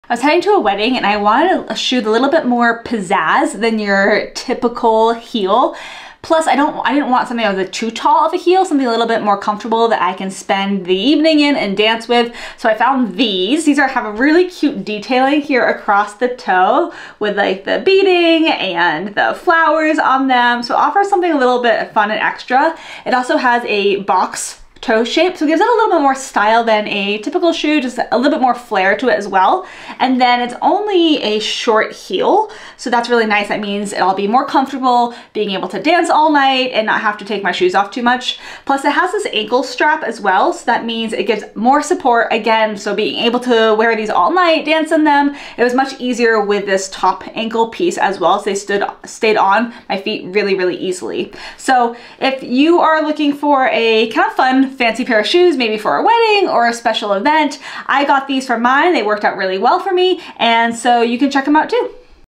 Where I ended up wearing these DREAM PAIRS Women's Wedding Heels
These Dream Pairs flower heels are great for special occasions like weddings!